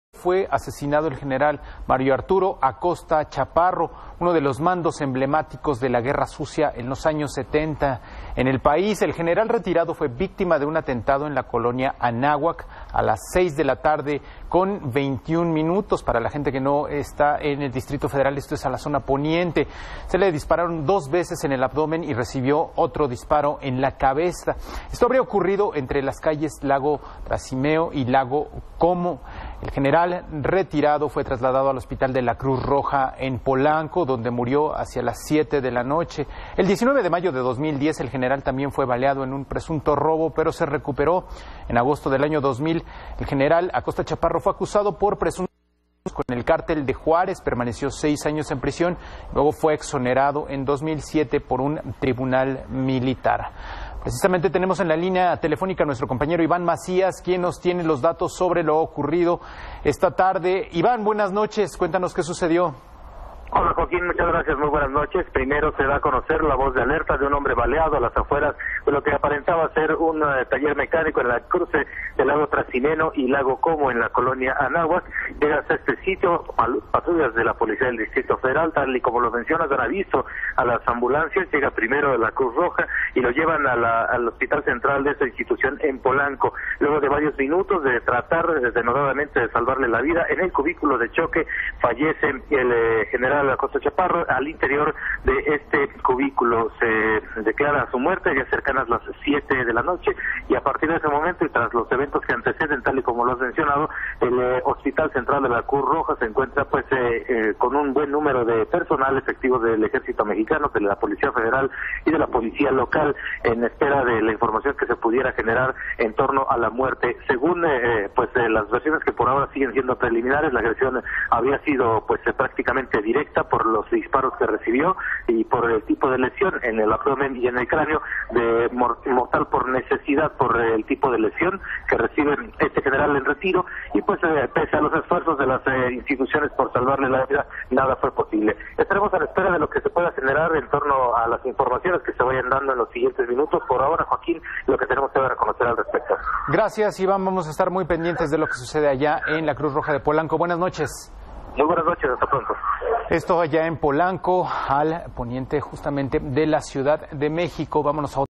Falleció de tres disparos el general Mario Arturo Acosta Chaparro
Tras seis años y 10 meses de permanecer prisionero en el Campo Militar Número Uno, el general Mario Arturo Acosta Chaparro obtuvo su libertad inmediata y absoluta en junio de 2007, luego de que las procuradurías general de la República y de Justicia Militar no pudieron acreditarle presuntos nexos con el narcotráfico.